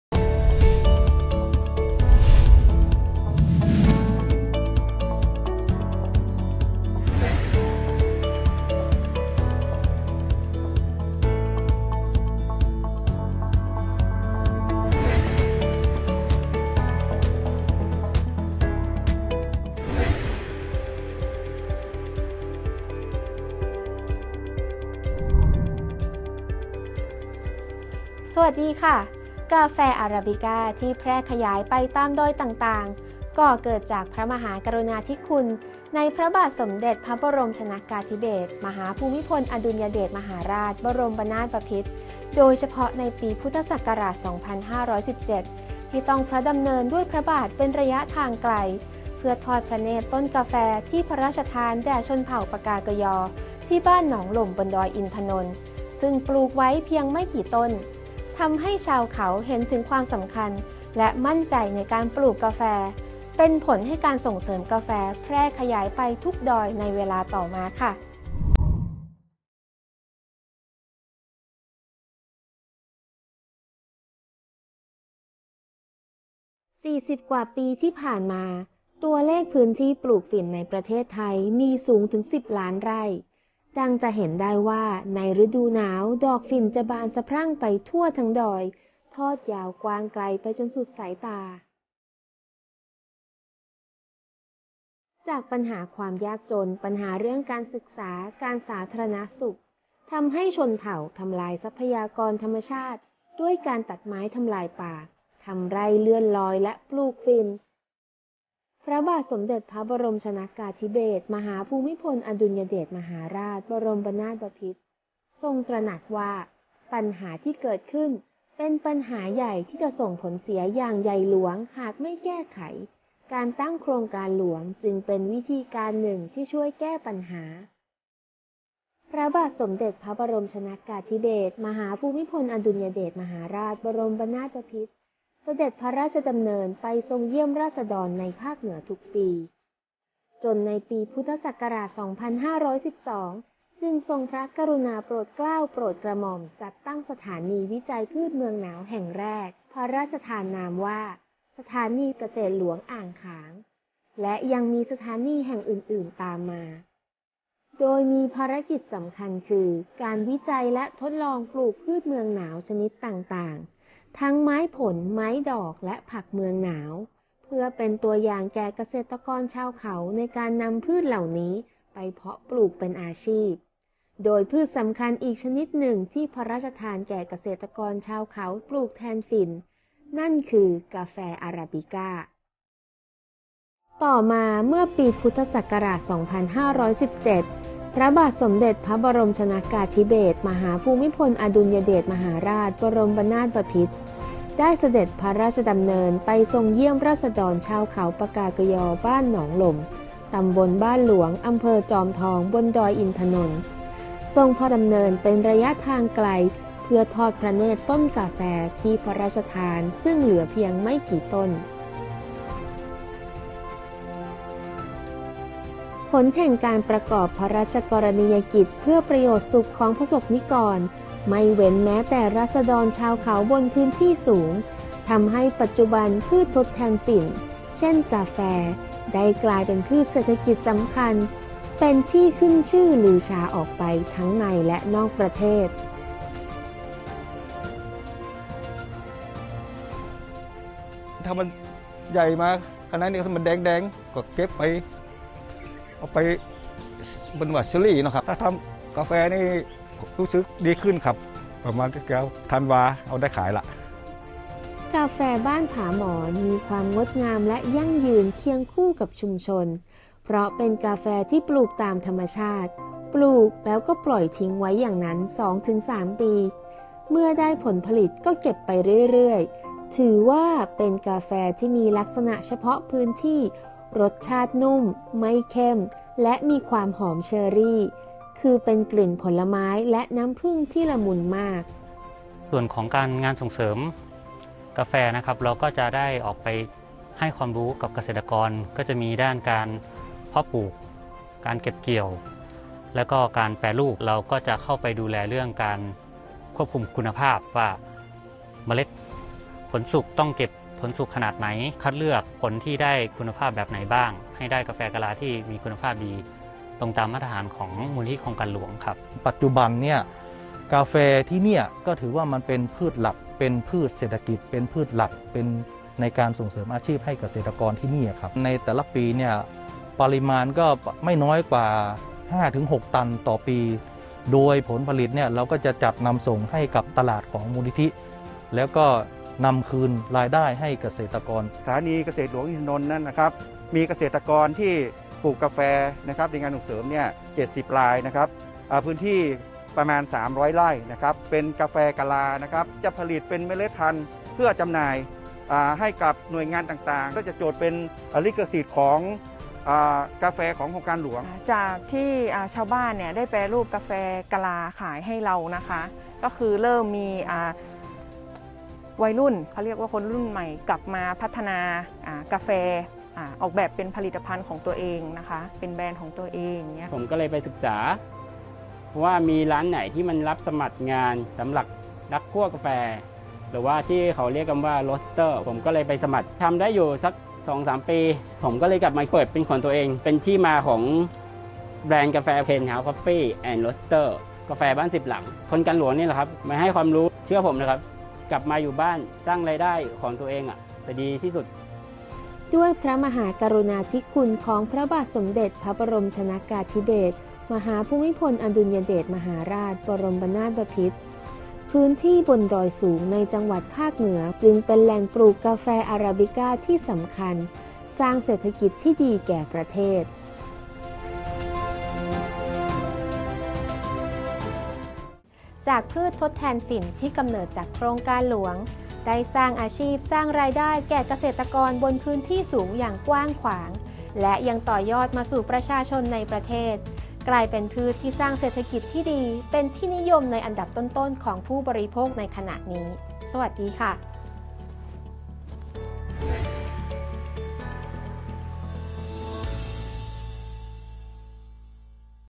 ตอนที่ 7 กาแฟก้าวดอยตามรอยพ่อ
กาแฟอะราบิกาที่แพร่ขยายไปตามดอยต่าง ๆ ก่อเกิดจากพระมหากรุณาธิคุณในพระบาทสมเด็จพระบรมชนกาธิเบศร มหาภูมิพลอดุลยเดชมหาราช บรมนาถบพิตร โดยเฉาะในปี พ.ศ. 2517 ที่ต้องทรงพระดำเนินเป็นระยะทางไกล เพื่อทอดพระเนตรต้นกาแฟที่พระราชทานแก่ชนเผ่าปกาเกอญอบ้านหนองหล่ม ในพื้นที่สถานีเกษตรหลวงอินทนนท์ ซึ่งปลูกไว้เพียงไม่กี่ต้น ทำให้ชาวเขาเห็นถึงความสำคัญและมั่นในในการปลุกกาแฟ เป็นผลให้การส่งเสริมกาแฟแพร่ขยายไปทุกดอยในเวลาต่อมา